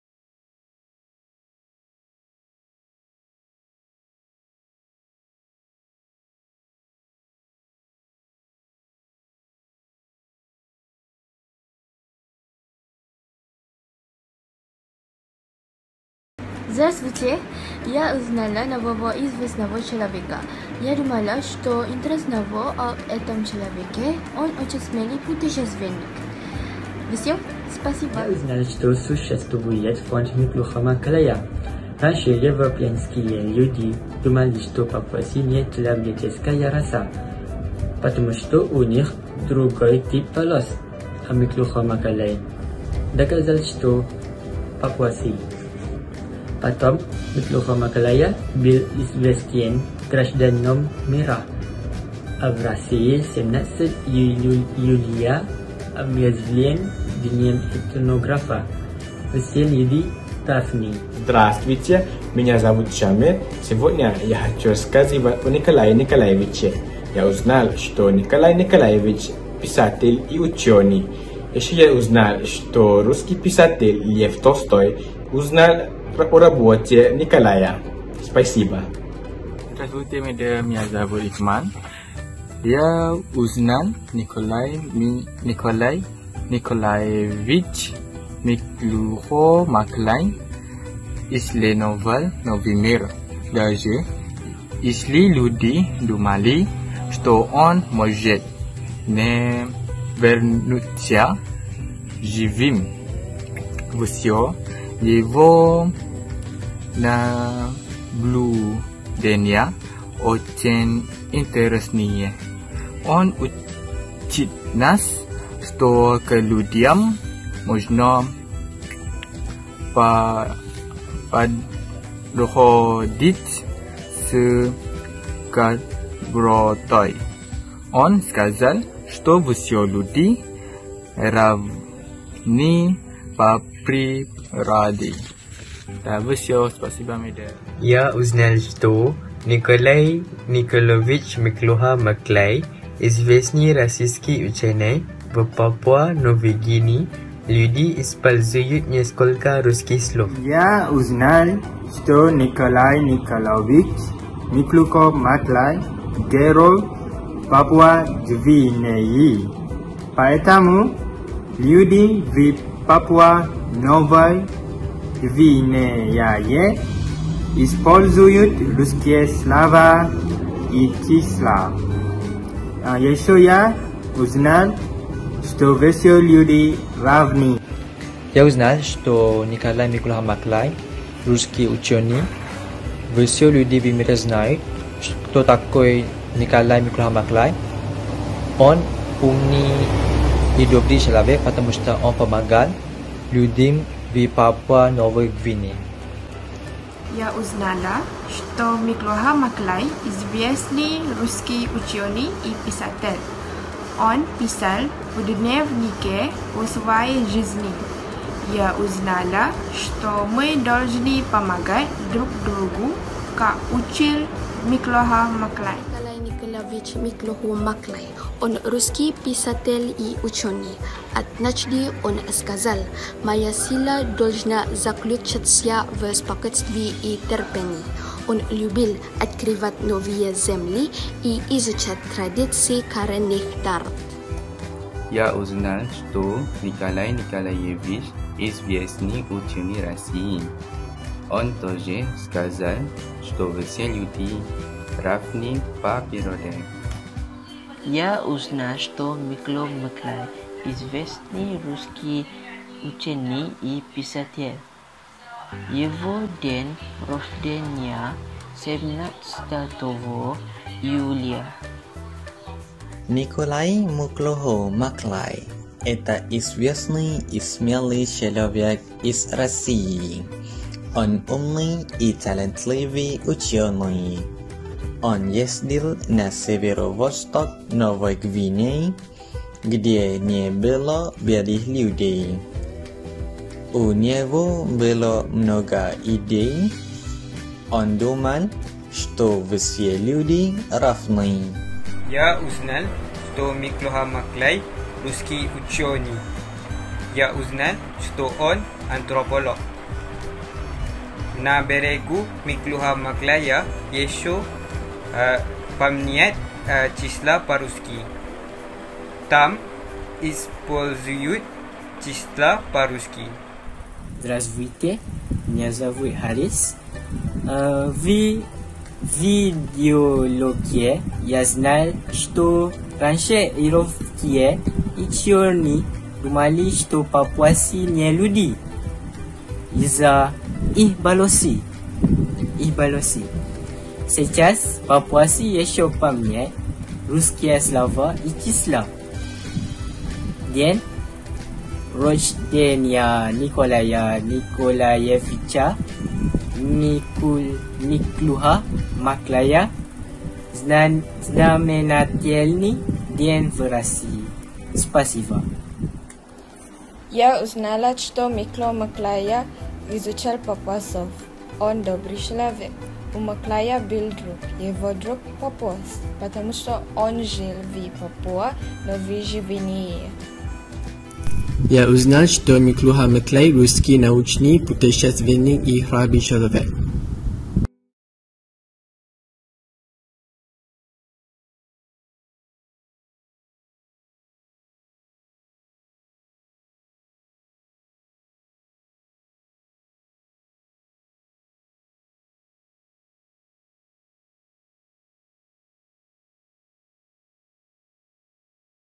Миклухо-Маклай в Малайзии
Видео подготовлено преподавателями РКИ и студентами международной программы МАИ-UniKL-MIAT по инициативе Российского центра науки и культуры в Куала-Лумпуре

Преподаватель русского языка: Полина Погадаева

Текст читали:
Muhammad Yusuf Faris Bin Omar,
Amirul Haikal Bin Adri Kamal,
Muhammad Haziq Imran Bin Khali Aziz,
Khoirul Ikhmal Bin Saidin,
Nurul Alya Binti Awang Basri,
Sufy Azyra Binti Mohd Azrie,
Ahmad Aliff Hilmi Bin Azahar,
Nurul Sakinah Binti Hishamuddin,
Mohamed Uwais Al-Qarni Bin Mohamed Syairoz,
Ahmad Amir Salihin Bin Addie Norazlie,
Muhammad Azfar Aiman Bin Salime,
Muhammad Syammil Faeez Bin Mohd Sani,
Nor Humaira Binti Mohamad Fadzlan,
Mohamad Hafizi Bin Akharuddin,
Mohamad Irfan Danial Bin Abdul Aziz,
Hariz Luqman Bin Mira Andika